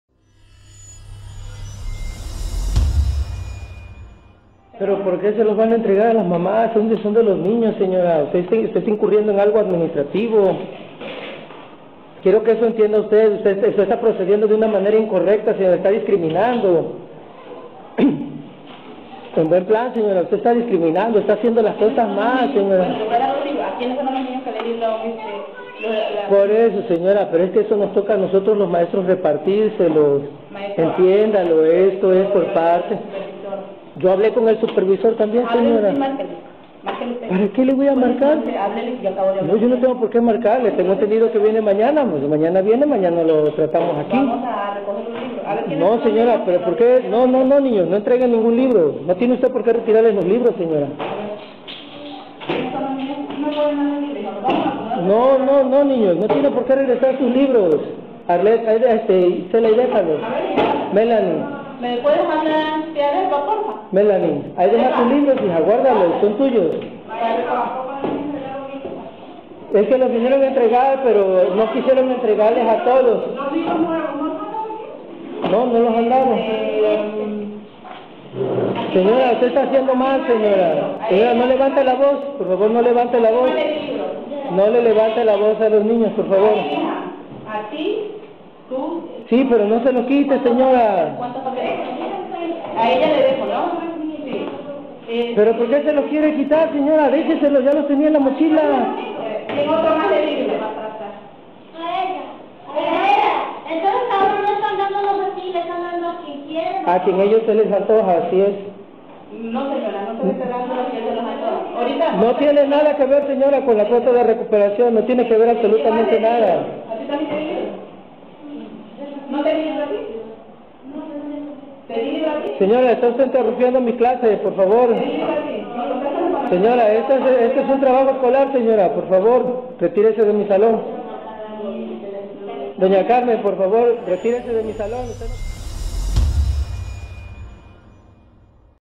Presidenta de padres de familia en Tlapacoyan quita los libros a menores
Presuntamente los padres de familia no habrían pagado la cuota de recuperación que exige la sociedad de padres de familia.